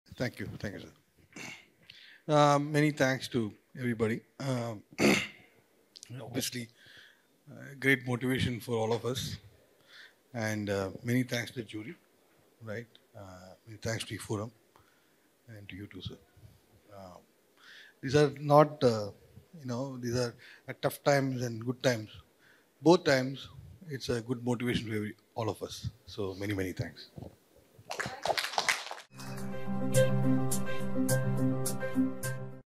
Prasanth Kumar, CEO, South Asia, GroupM addresses the audience at e4mMediaAce Awards 2023
Prasanth Kumar, CEO, South Asia, GroupM holding the prestigious e4mMediaAce 'Network of the Year Award', addresses the audience, and expresses Thanks ❤️ 👏